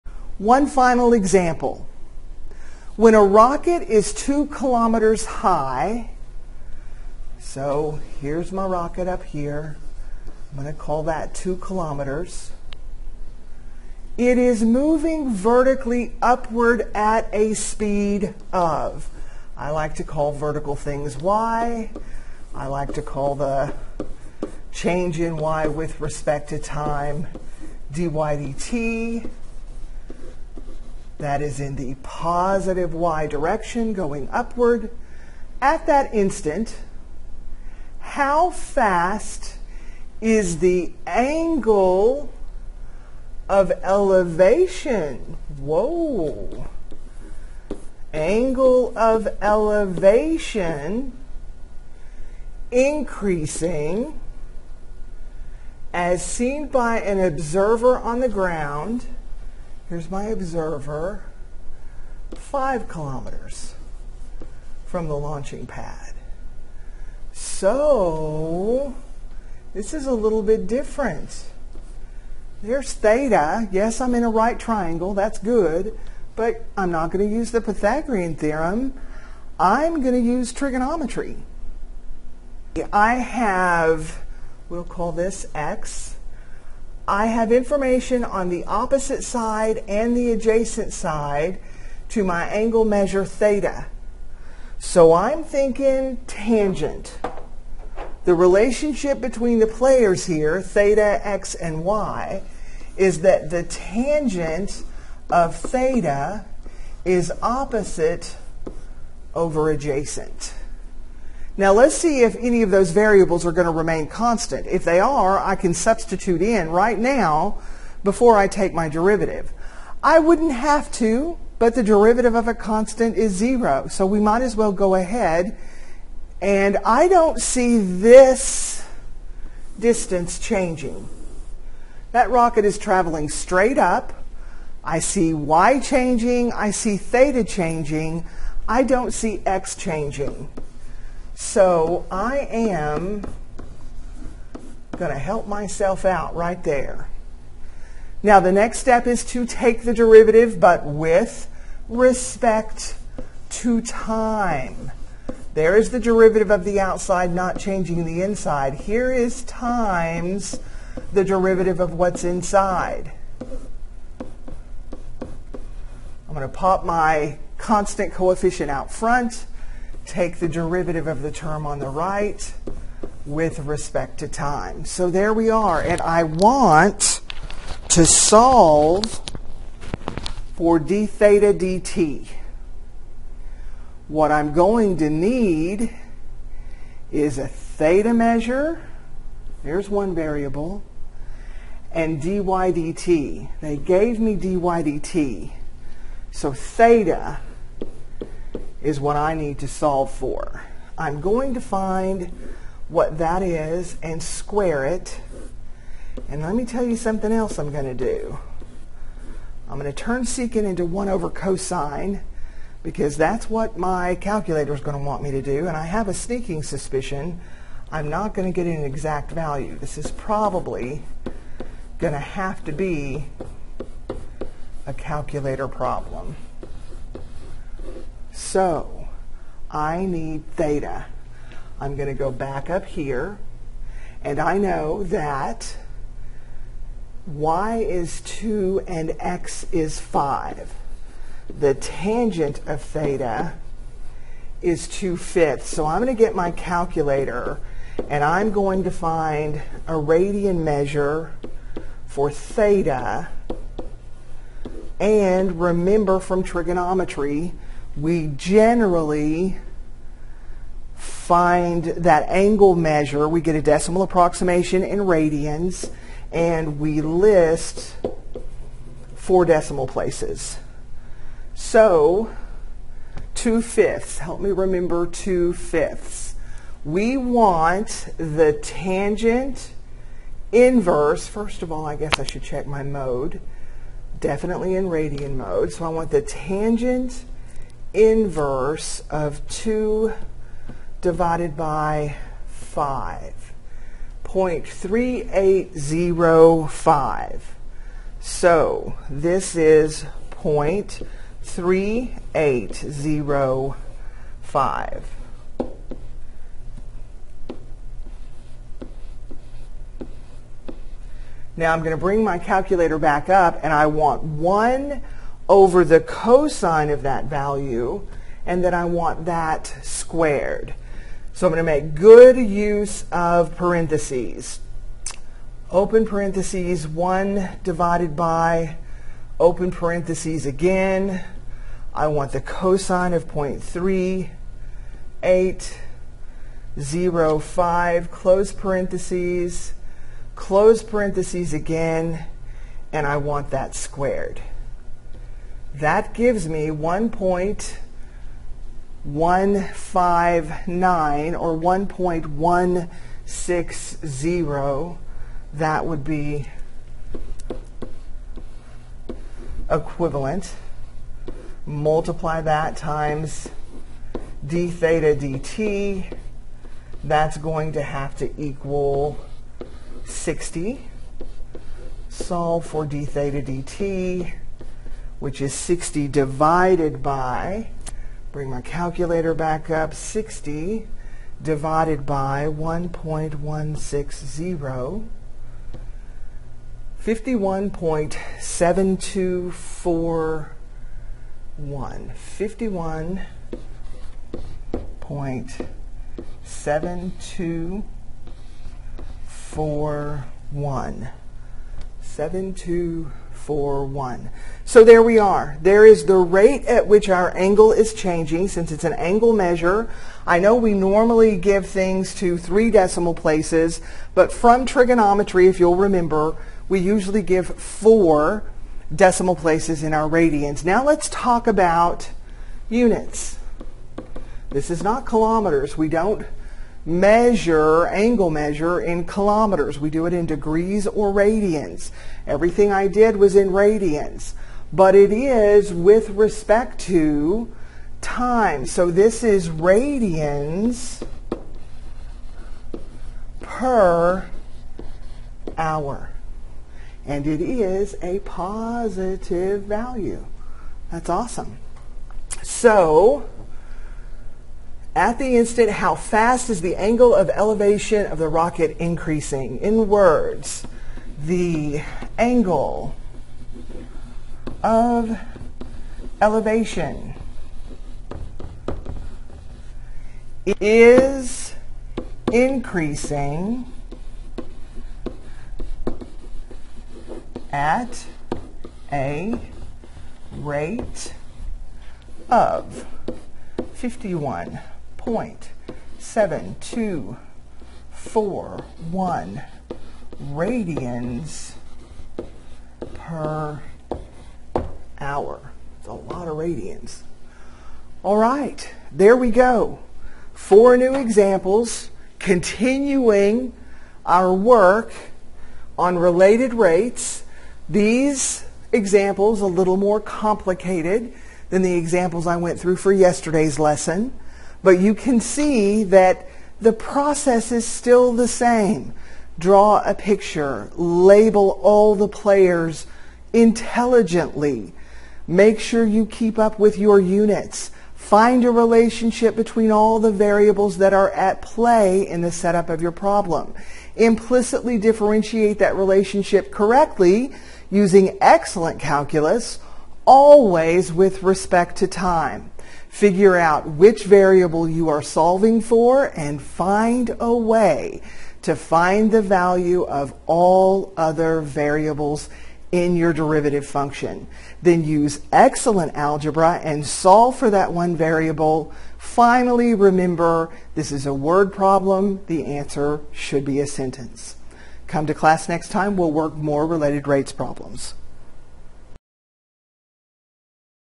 Related Rates Continued 3
We work a right triangle example that is different from the others we have seen.